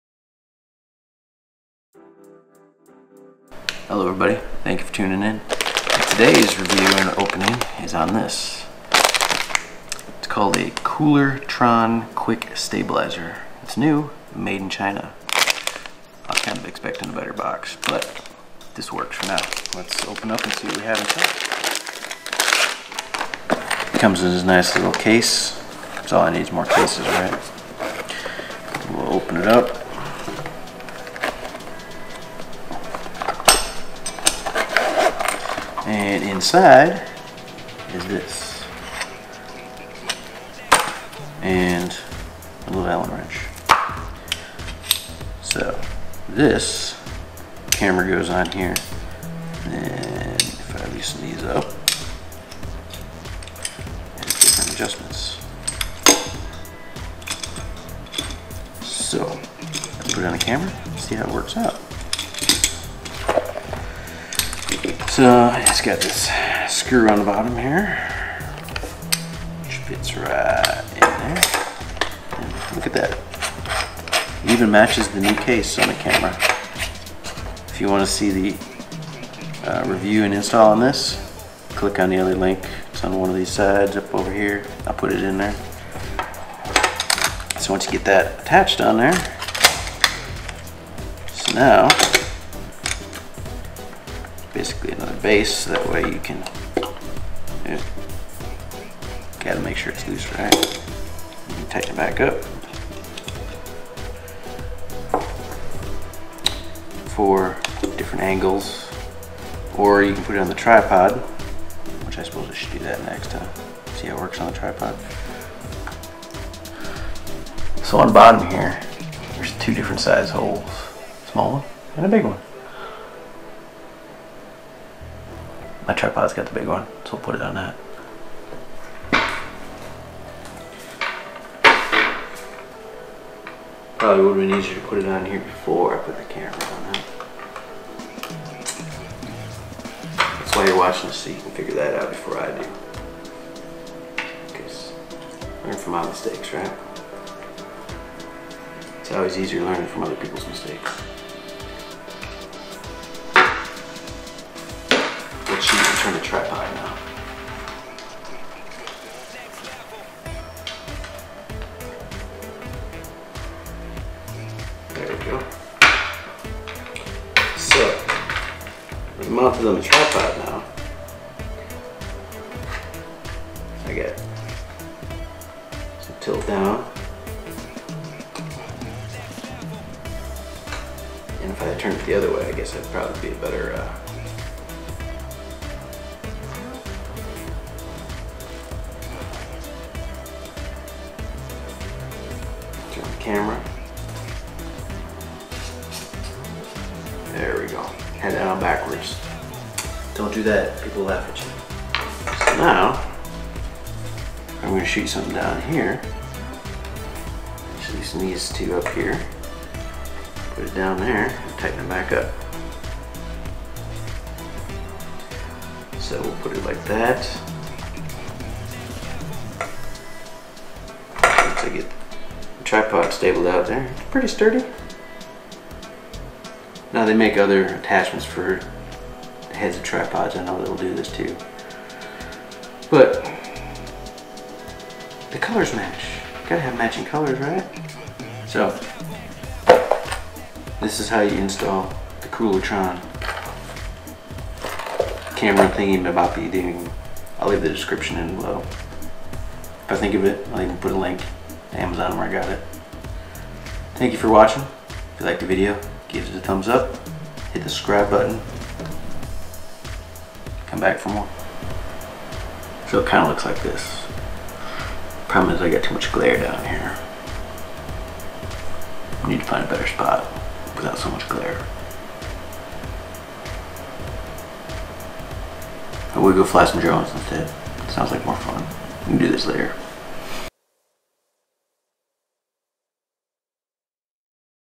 Koolertron Folding Z Flex Tilt Head Camera Bracket
Koolertron Quick Release Plate Aluminum Folding Z Flex Tilt Head Camera Bracket, Compatible with Slide Rail Camera Camcorder Tripod Stabilizers

I bought this one because I read the reviews about it being sturdy and didn't want anything cheap to hold my expensive camera.

Awesome Superior build Quality.  The knobs turn and the device tightens up very well to hold your camera in place.

Material: CNC Machined Aluminum alloy
Standard Load-bearing: 3kg / 6.6lbs
Attachment (top): 1/4" male thread,
(bottom): 1/4" 3/8" female thread
Weight: 400g/0.88lb